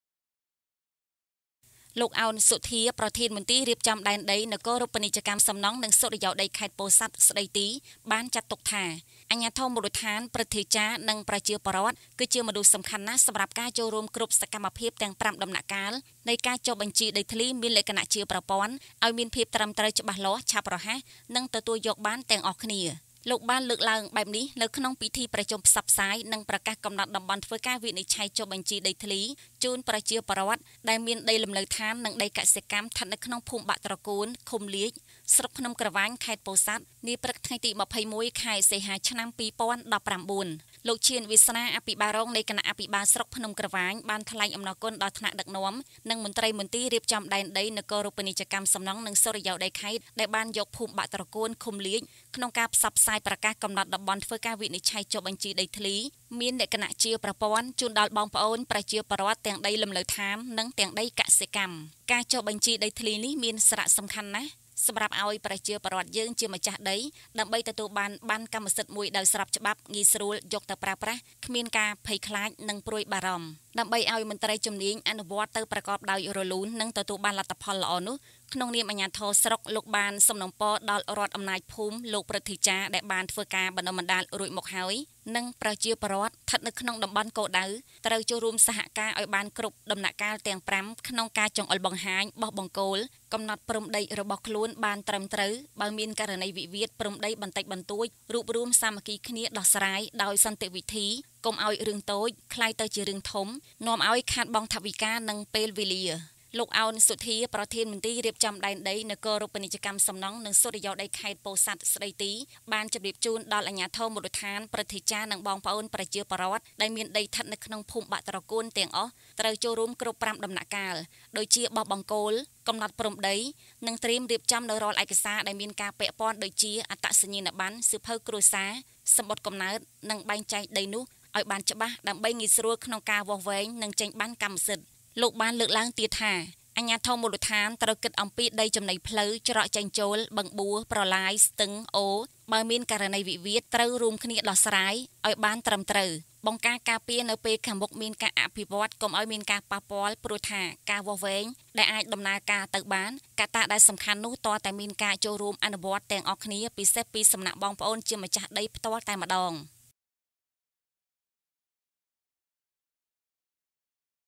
ពិធីប្រជុំផ្សព្វផ្សាយ និងប្រកាសកំណត់តំបន់ធ្វើការវិនិច្ឆ័យចុះបញ្ជីដីធ្លីជូនប្រជាពលរដ្ឋ
លោក ឱន សុទ្ធា ប្រធានមន្ទីររៀបចំដែនដី នគរូបនីយកម្ម សំណង់និងសុរិយោដីខេត្ត ខេត្តពោធិ៍សាត់ស្តីទី បានចាត់ទុកថា អាជ្ញាធរមូលដ្ឋាន ព្រឺទ្ធាចារ្យនិងប្រជាពលរដ្ឋ គឺជាមនុស្សសំខាន់ណាស់ សម្រាប់ការចូលរួមគ្រប់សកម្មភាពទាំង៥ដំណាក់កាលនៃការ ចុះបញ្ជីដីធ្លីមានលក្ខណៈជាប្រព័ន្ធឲ្យមានភាពត្រឹមត្រូវ ច្បាស់លាស់ ឆាប់រហ័សនិងទទួលយកបានទាំងអស់គ្នា។
        លោកបានលើកឡើងបែបនេះនៅក្នុងពិធីប្រជុំ ផ្សព្វផ្សាយនិងប្រកាសកំណត់តំបន់ធ្វើការវិនិច្ឆ័យចុះបញ្ជីដីធ្លីជូន ប្រជាពលរដ្ឋ ដែលមានដីលំនៅដ្ឋាននិងដីកសិកម្ម ស្ថិតក្នុងភូមិបាក់ត្រគួន ឃុំលាច ស្រុកភ្នំក្រវាញ ខេត្តពោធិ៍សាត់ នាព្រឹកថ្ងៃទី២១ខែសីហាឆ្នាំ ២០១៩ ។
          លោក ឈាន វាសនា អភិបាលរងនៃគណៈអភិបាលស្រុកភ្នំក្រវាញ បានថ្លែងអំណរគុណដល់ថ្នាក់ដឹកនាំ និងមន្ត្រីមន្ទីររៀបចំដែនដី នគរូបនីយកម្ម សំណង់និងសុរិយោដីខេត្ត ដែលបានយក ភូមិបាក់ត្រគួនឃុំលាច ក្នុងការផ្សព្វផ្សាយប្រកាស កំណត់តំបន់ធ្វើការវិនិច្ឆ័យចុះបញ្ជីដីធ្លីមានលក្ខណៈជាប្រព័ន្ធជូនដល់បងប្អូន ប្រជាពលរដ្ឋទាំងដីលំនៅដ្ឋាន និងទាំងដីកសិកម្ម។ ការចុះបញ្ជីដីធ្លី នេះមានសារៈសំខាន់ណាស់សម្រាប់ឲ្យប្រជាពលរដ្ឋយើងជាម្ចាស់ដី ដើម្បីទទួលបានបណ្ណ័កម្មសិទ្ធិមួយដោយស្របច្បាប់ ងាយស្រួល យកទៅប្រើប្រាស់ គ្មានការភ័យខ្លាចនិងព្រួយបារម្ម។ ដើម្បីឲ្យមន្រ្តីជំនាញអនុវត្តទៅប្រកបដោយរលូននិងទទួលបានលទ្ធផលល្អនោះ ក្នុងនាមអាជ្ញាធរស្រុកលោកបានសំណូមពរដល់រដ្ឋអំណាចភូមិ លោកព្រឹទ្ធាចារ្យ ដែលបាន ធ្វើការបណ្តុះបណ្តាលរួចមកហើយនិងប្រជាពលរដ្ឋស្ថិតនៅក្នុងតំបន់គោលដៅ ត្រូវចូលរួមសហការ ឲ្យបានគ្រប់ដំណាក់កាលទាំង៥ ក្នុង ការចង្អុលបង្ហាញ បោះបង្គោល កំណត់ព្រំដីរបស់ខ្លួនបានត្រឹមត្រូវ បើមានករណី វិវាទព្រំដីបន្តិចបន្តួច រួបរួមសាមគ្គីគ្នាដោះ ស្រាយ ដោយ សន្តិវិធីកុំឲ្យ រឿងតូច ក្លាយទៅជារឿងធំនាំឲ្យខាតបង់ថវិកានិងពេលវេលា។
 លោក ឱន សុទ្ធាប្រធានមន្ទីររៀបចំដែនដី នគរូបនីយកម្ម សំណង់និងសុរិយោដីខេត្តពោធិ៍សាត់ស្តីទី បានជម្រាបជូនដល់ អាជ្ញាធរមូលដ្ឋាន ព្រឹទ្ធាចារ្យនិង បងប្អូន ប្រជាពលរដ្ឋដែលមានដីស្ថិតនៅក្នុងភូមិបាក់ត្រគួនទាំងអស់ ត្រូវចូលរួមគ្រប់៥ដំណាក់កាល ដូចជាបោះបង្គោល កំណត់ព្រំដី និងត្រៀមរៀបចំនូវរាល់ឯក សារ ដែលមានការពាក់ព័ន្ធដូចជា អត្តសញ្ញាណបណ្ណ័ សៀវភៅគ្រួសារ សំបុត្រកំណើត និងបែងចែកដីនោះឲ្យបានច្បាស់ដើម្បីងាយស្រួលក្នុងការវាស់វែងនិងចេញប័ណ្ណកម្មសិទ្ធិ ។ លោកបានលើកឡើងទៀតថា អាជ្ញាធរមូលដ្ឋានត្រូវគិតអំពីដីចំណីផ្លូវ ច្រកចេញចូល បឹងប៊ួ ប្រឡាយ ស្ទឹង អូរ បើមានករណីវិវាទត្រូវរួមគ្នាដោះស្រាយឲ្យ បានត្រឹមត្រូវ បង្ការការពារនៅពេល ខាងមុខមានការអភិវឌ្ឍន៍កុំឲ្យមានការប៉ះពាល់ព្រោះថា ការវាស់វែង ដែលអាច ដំណើរការ ទៅ បាន កត្តាដែលសំខាន់នោះទាល់តែមានការចូលរួមអនុវត្តទាំងអស់គ្នា ពិសេសពីសំណាក់ បងប្អូនជាម្ចាស់ដី ផ្ទាល់តែម្តង ។
ស្ងួន ឈឿន ពោធិ៍សាត់